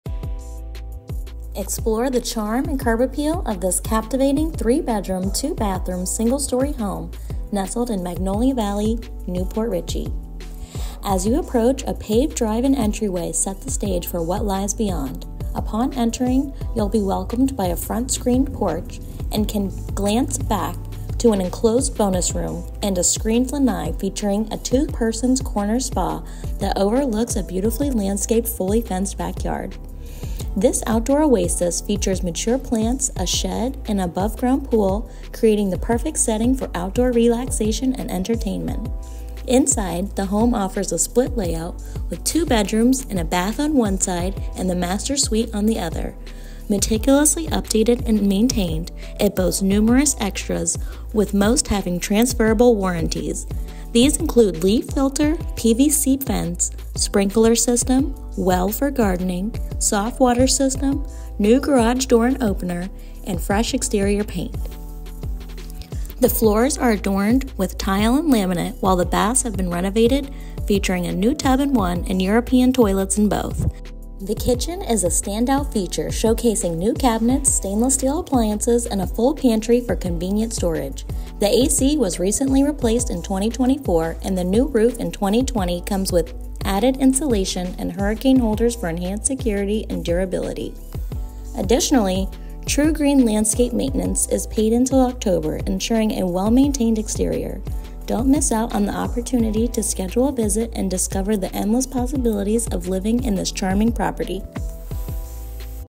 CHARMING 3 BED/2 BATH HOME | 7924 DATURA LN, NEW PORT RICHEY, FL 34653
Explore the charm and curb appeal of this captivating 3-bedroom, 2-bathroom single-story home nestled in Magnolia Valley, New Port Richey. As you approach, a paved drive and entryway set the stage for what lies beyond. Upon entering, you'll be welcomed by a front screened porch and can glance a back enclosed bonus room (12x16) and a screened lanai featuring a 2-persons' corner spa, that overlooks a beautifully landscaped fully fenced backyard. This outdoor oasis features mature plants, a shed, and an above-ground pool, creating the perfect setting for outdoor relaxation and entertainment. Inside, the home offers a split layout, with two bedrooms and a bath on one side, and the master suite on the other. Meticulously updated and maintained, it boasts numerous extras with most having transferable warranties. These include Leaf Filter, PVC fence, sprinkler system, well for gardening, soft water system, new garage door and opener, and fresh exterior paint. The floors are adorned with tile and laminate, while the baths have been renovated, featuring a new tub in one and European toilets in both. The kitchen is a standout feature, showcasing new cabinets, stainless steel appliances, and a full pantry for convenient storage. The A/C was recently replaced in 2024, and the new roof in 2022 comes with added insulation and hurricane holders for enhanced security and durability. Additionally, True Green landscape maintenance is paid until October, ensuring a well-maintained exterior.

FOR MORE INFO ON LIVING IN NEW PORT RICHEY AND SURROUNDING COMMUNITIES

00:00 Introduction
00:36 Bedroom
00:48 Hard Wood Floors